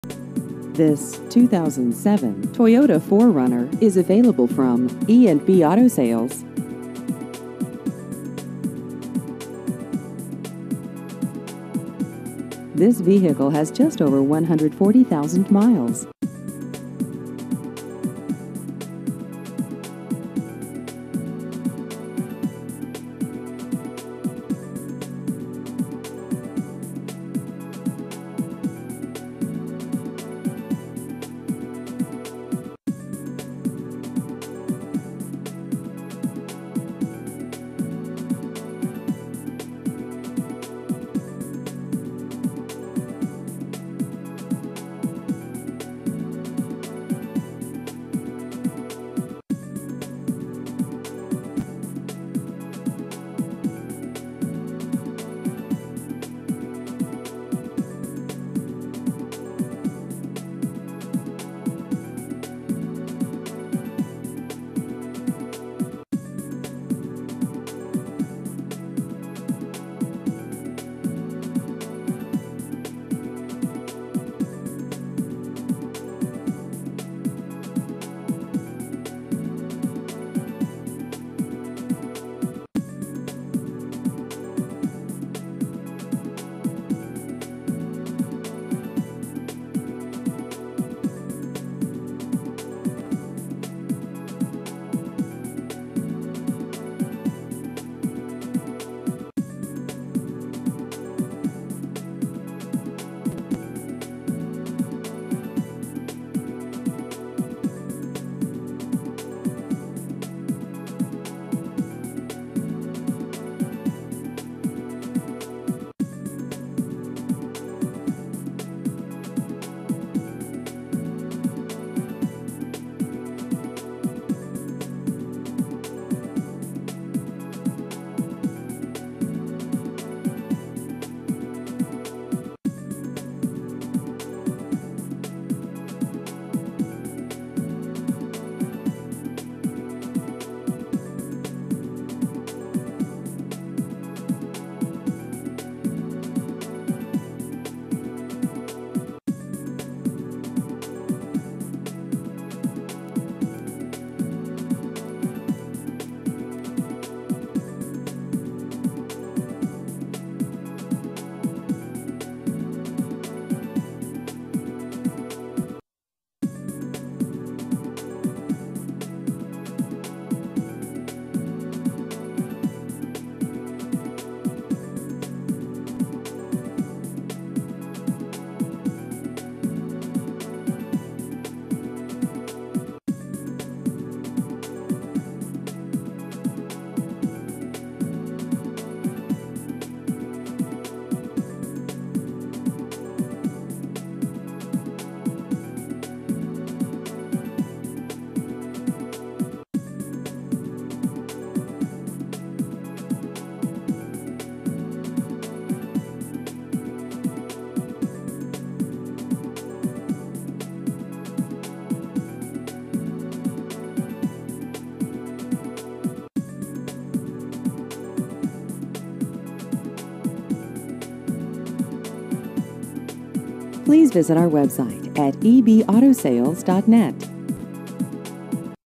2007 Toyota 4Runner Used Cars Albuquerque NM
This 2007 Toyota 4Runner is available from E & B Auto Sales.